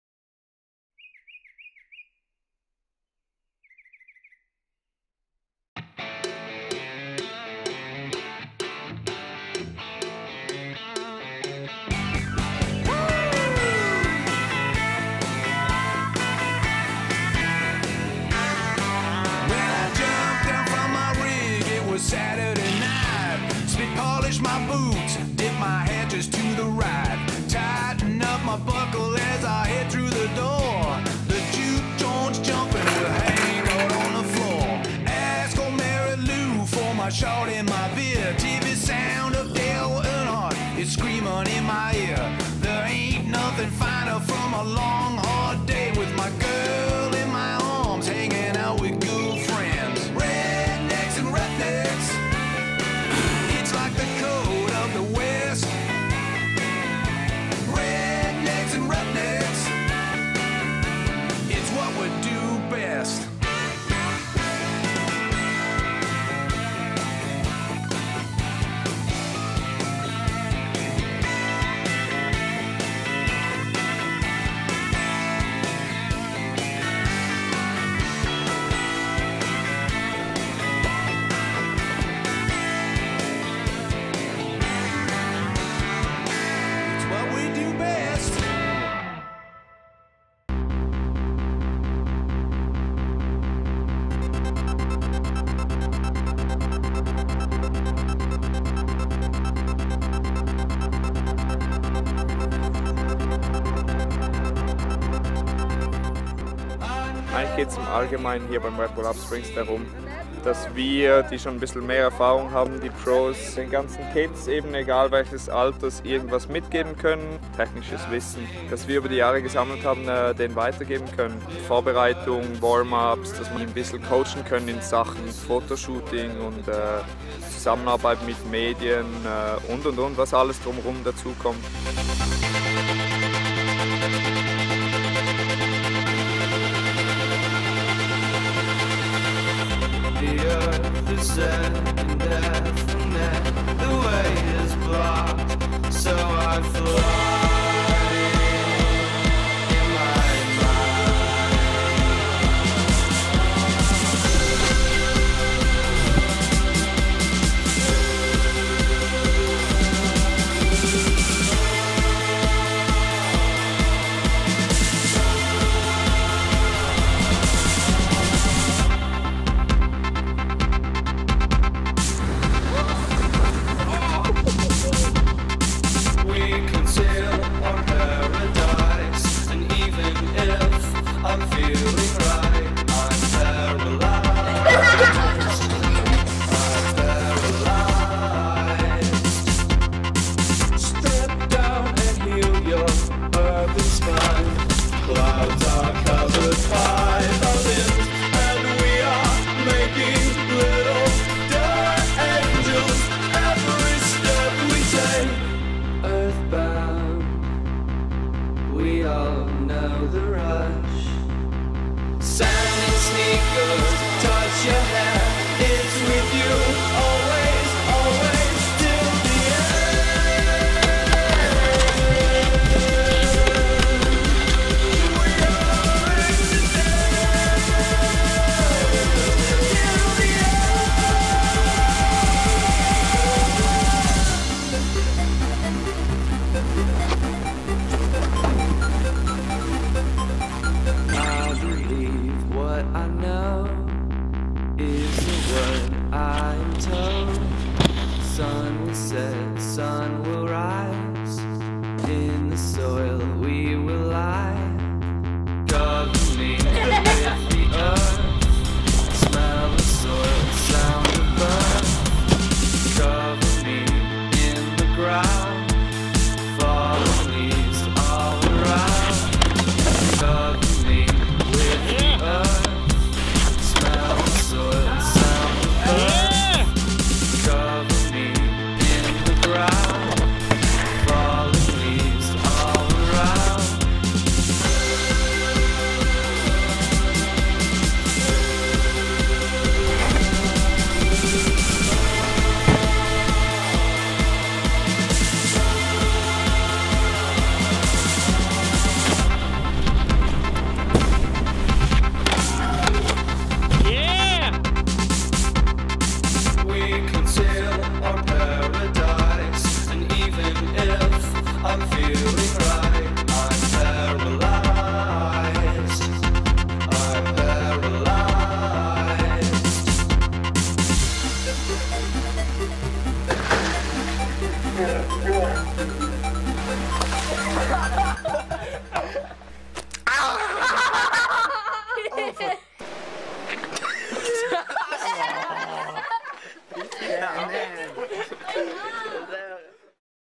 Snowboard Rookies Ride with the Pros - Red Bull Upsprings 2012
For the fourth time, Snowboard-Rookies from Germany, Austria and Switzerland get the chance to "live and ride" with Snowboard-Pros.This time in Oberstdorf, Germany